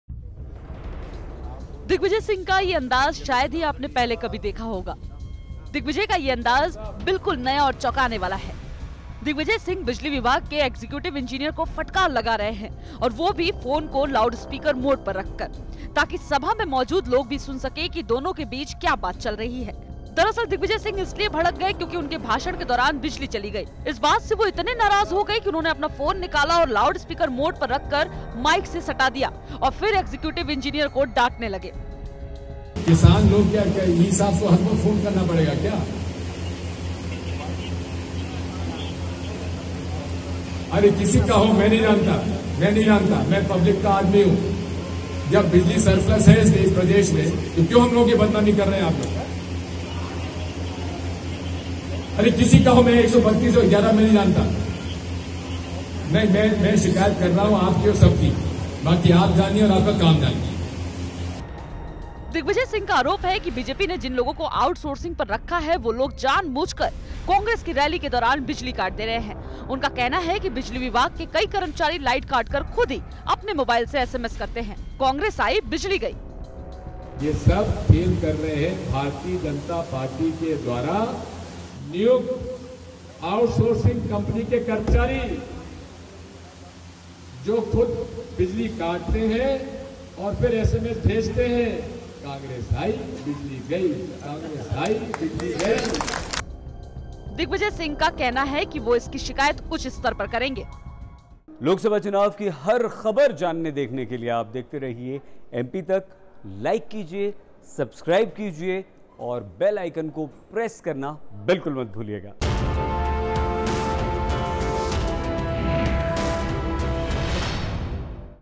दिग्विजय सिंह के भाषण के दौरान बिजली कट की जिम्मेदार बीजेपी !| MP Tak
दिग्विजय सिंह के भाषण के दौरान अचानक बिजली चली गई...जिसके बाद दिग्विजय सिंह ने तत्काल बिजली विभाग के कर्मचारी पर फोन लगाया...और उसको मंच से ही हड़काया...